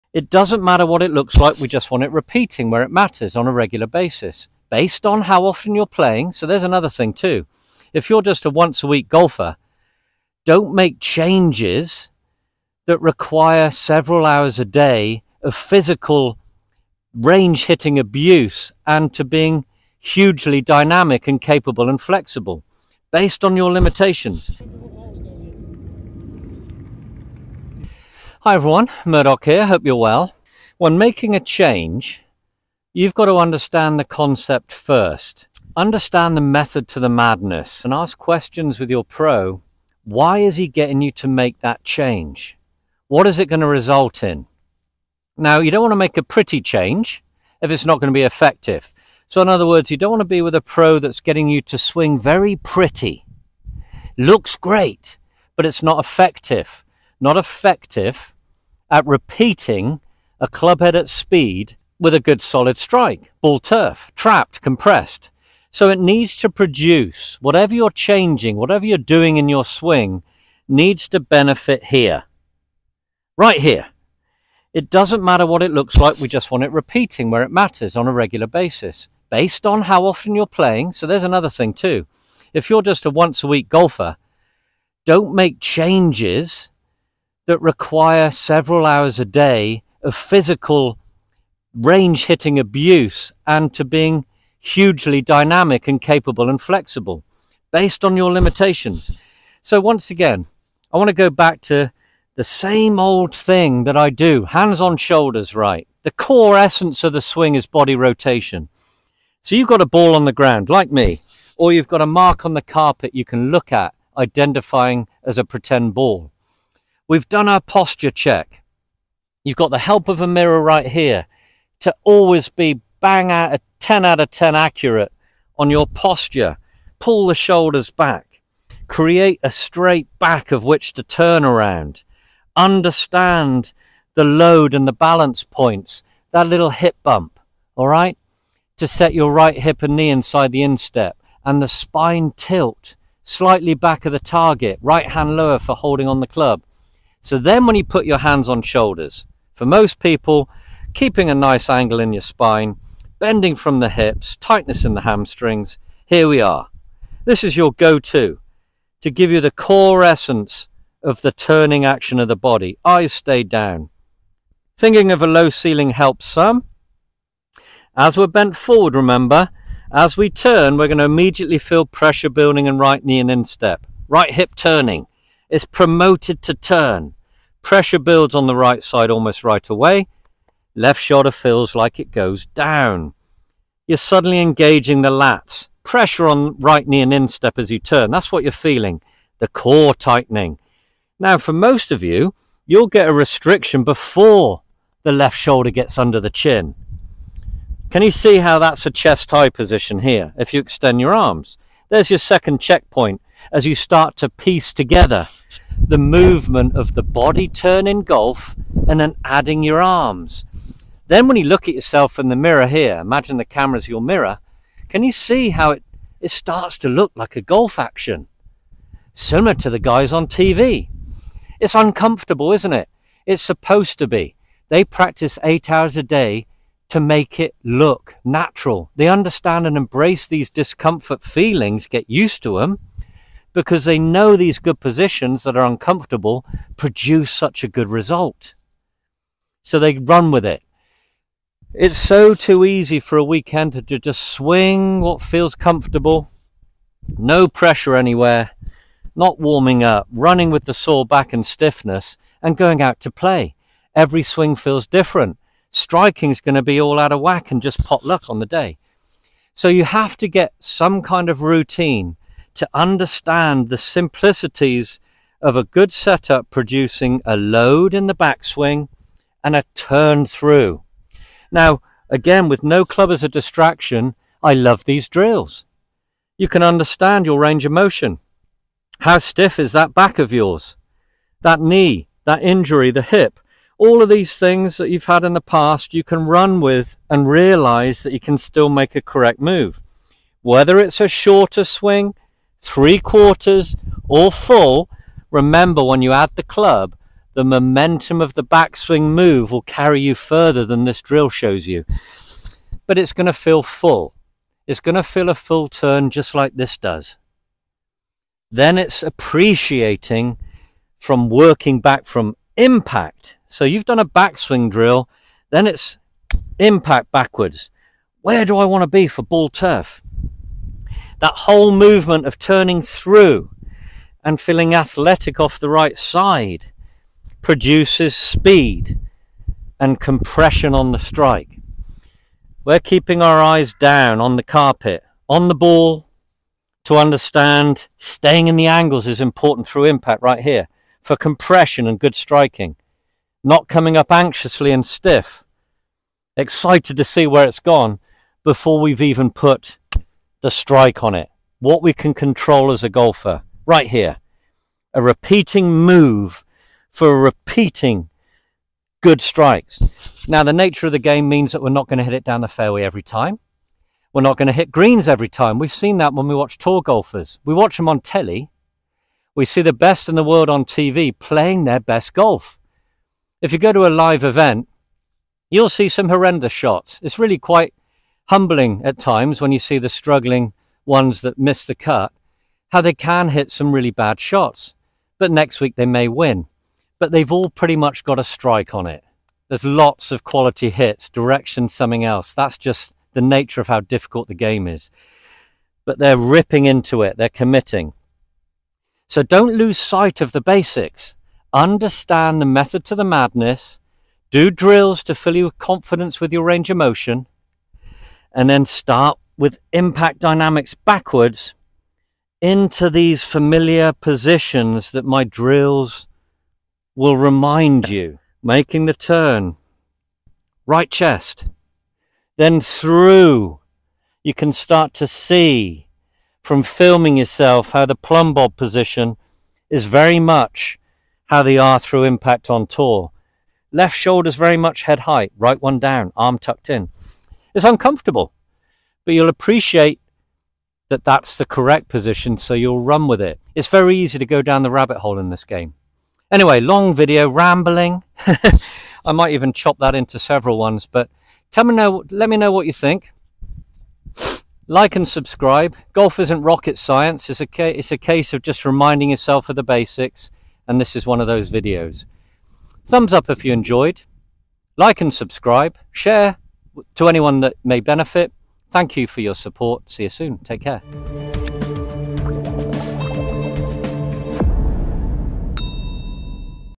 Seniors and Weekend Golfers - All You Need To Know
To be consistent at golf you need to stay consistent with your mind set and understandings of your physical self.  Next?  Impact dynamics then working backwards from that.

This video will give you a reminder to ask yourself “why are you working on that?” and to check your limitations on a regular basis.

I hope you enjoy and benefit.  Thank you for your support! 🙏🏼